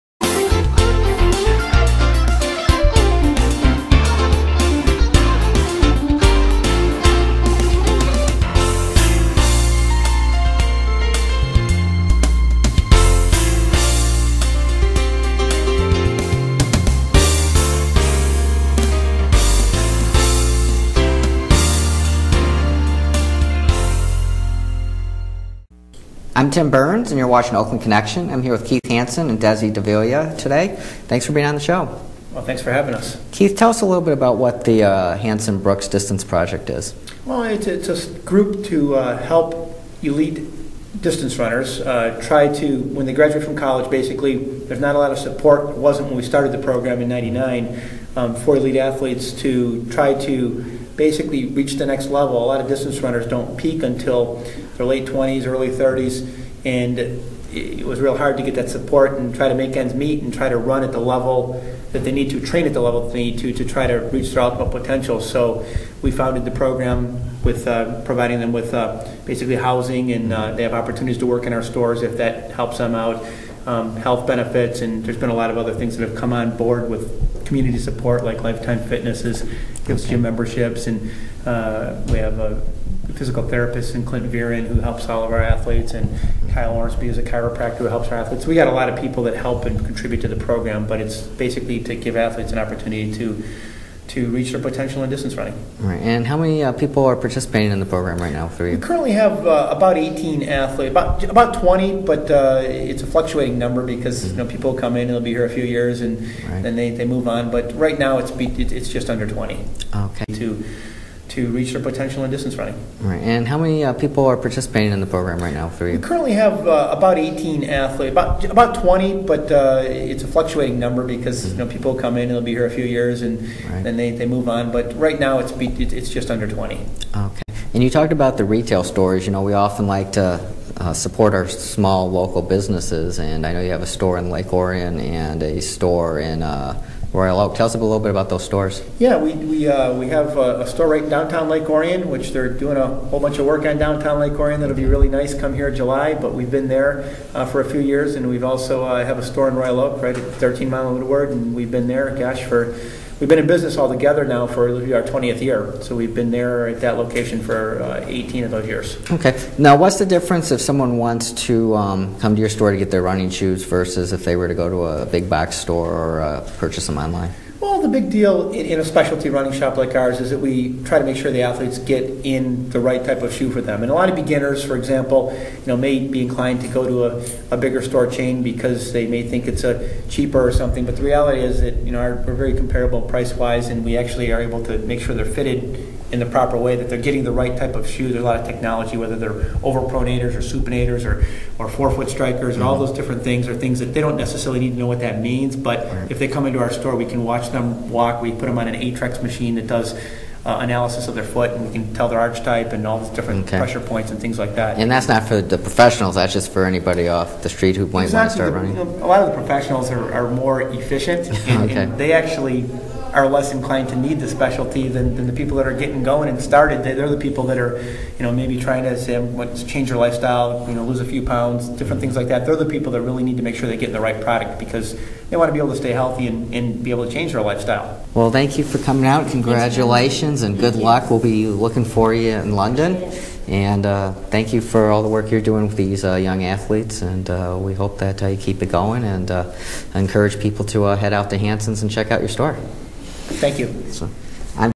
Keith Hanson
Professional running coach Keith Hanson talks about the Hanson-Brooks Running Project